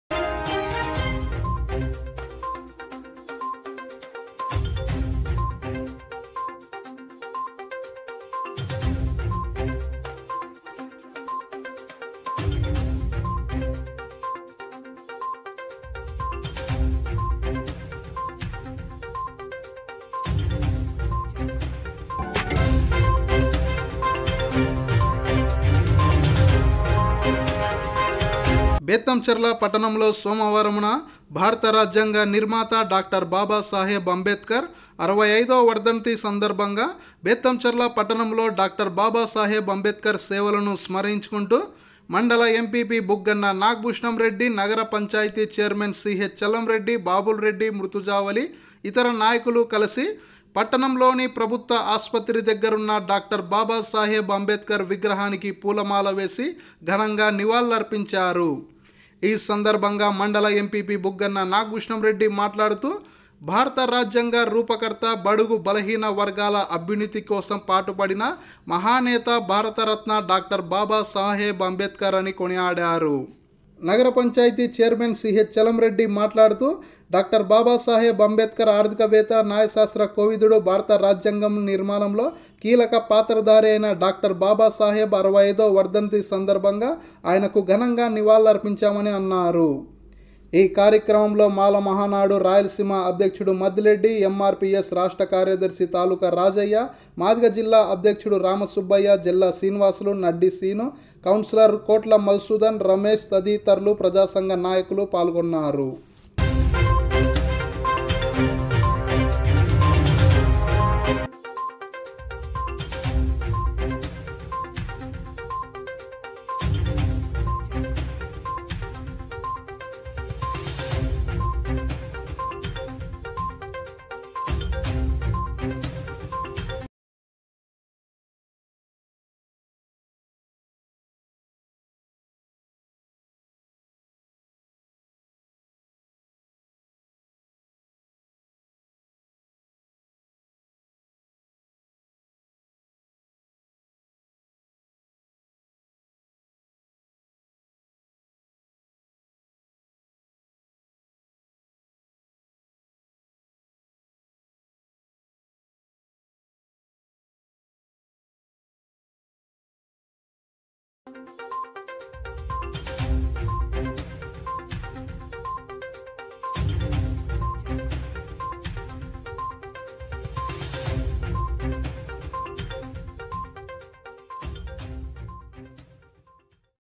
బేతంచర్లలో డా.బి.ఆర్ అంబేద్కర్ వర్ధంతి సందర్భంగా నివాళులర్పించిన YSRCP నాయకులు,MRPSమాలమహానాడునాయకులు
సోమవారంన బేతంచర్లలో డా.బి.ఆర్.అంబేద్కర్ 65వ వర్ధంతి సందర్భంగా ఘనంగా నివాళులర్పించిన వైఎస్సార్ సీపీ నాయకులు మాలమహానాడు,ఎమ్మార్పీఎస్ ప్రజాసంఘాలు.